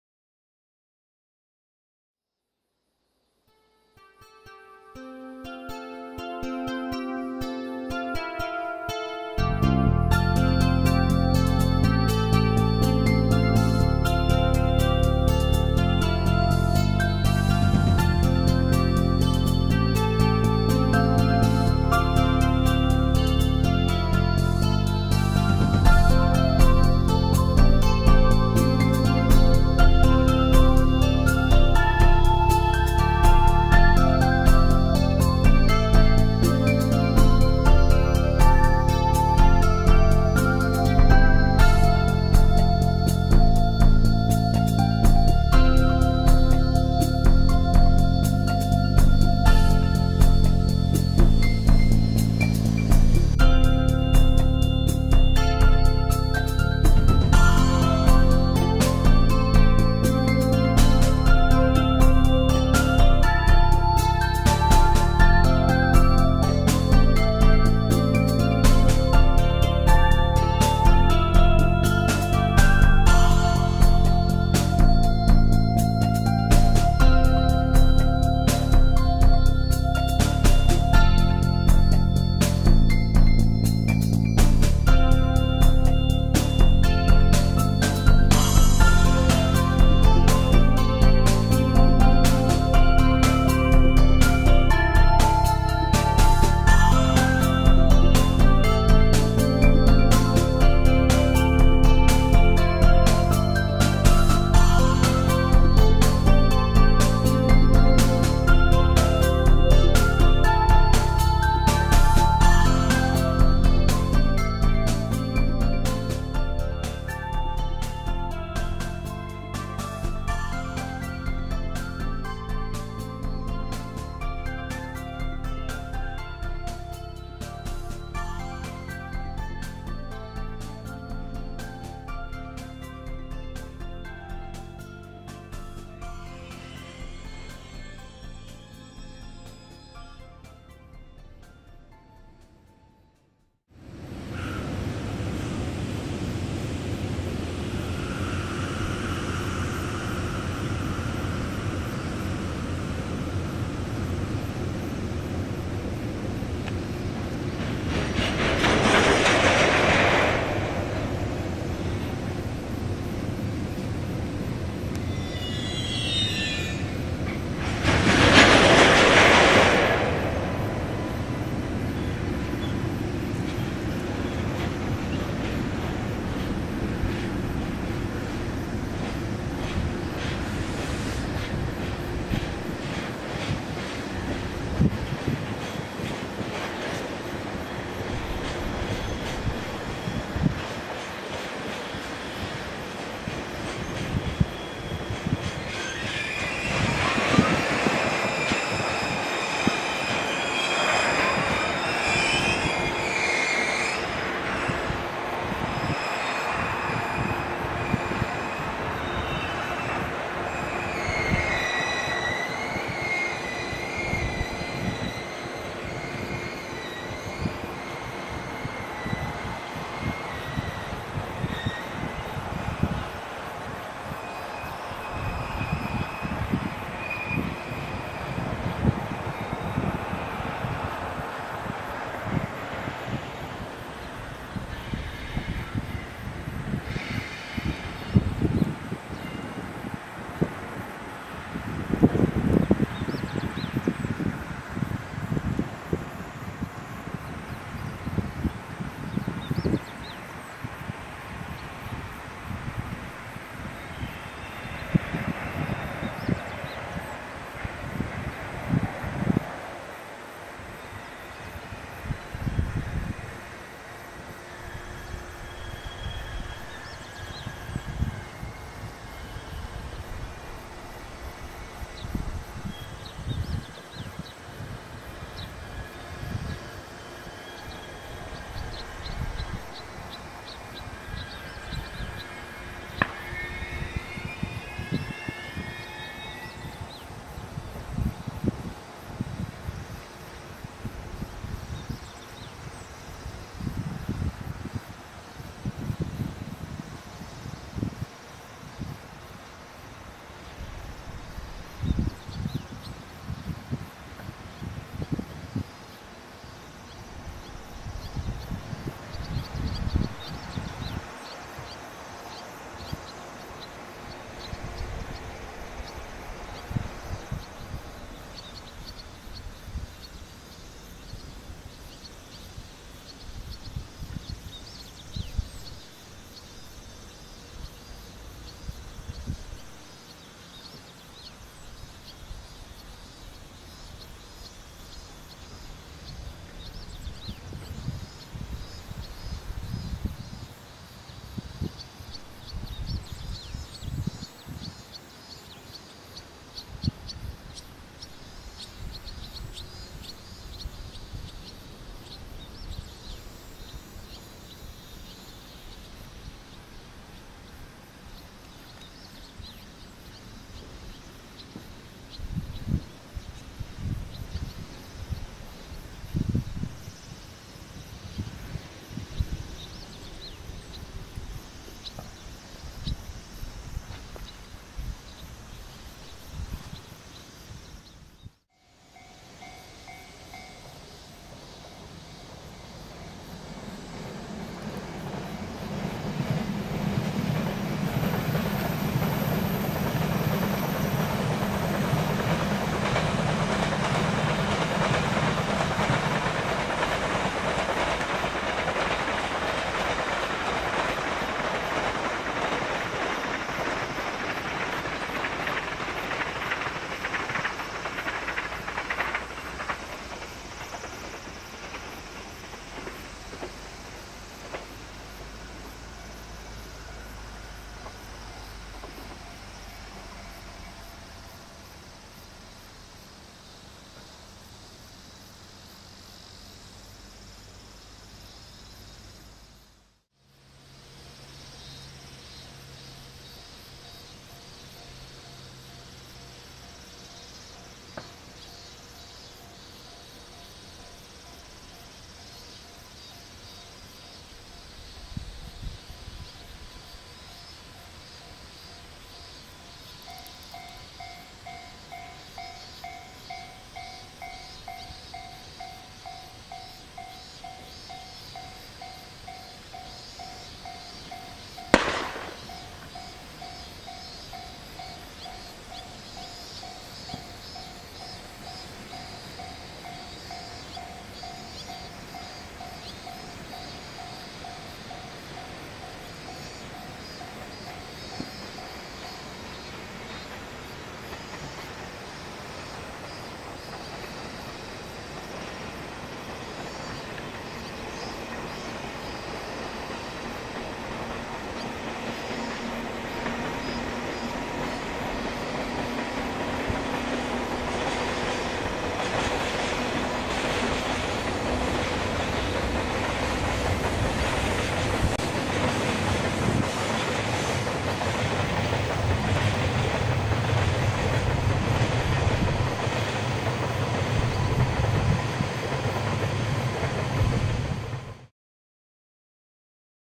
姨捨駅 EH200石油輸送返空 スイッチバック Aug.-Sep., 2013
姨捨駅で、EH200が牽引する石油輸送の返空列車がスイッチバックし、待避する様子です。
8月から9月にかけて、姨捨駅界隈に通い2084ﾚ、5462ﾚのカットを集め編集しました。
撮影日が複数であるため、車番、編成両数などは統一されていません。

あわせて姨捨駅からの風景もお楽しみ下さい。

撮影にはCanon iVIS HF R32、Nikon D7000、iPhone5を、編集にはTMPGEnc VMW5を使っています。